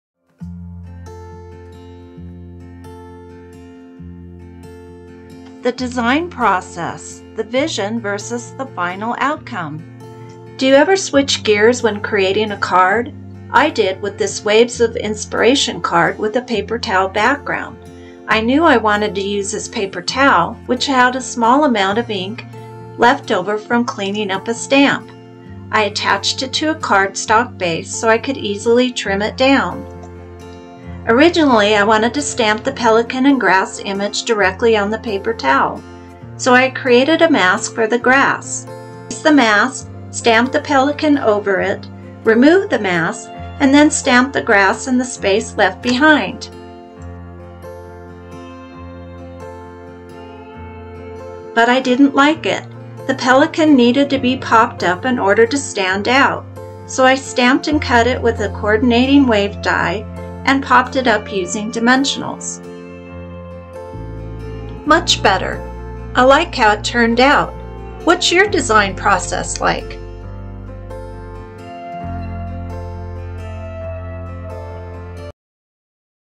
The Design Process-It's ok to Switch Gears!
Do you ever have a card creation in mind but switch design gears halfway through the process?
I do.  It was the case with this Paper Towel Background Waves of Inspiration Card.
See how I did a pivot with the final design!
Anne Hayward, Independent Stampin' Up! Demonstrator
Order Stampin' Up! product online 24/7:
annehayward.stampinup.net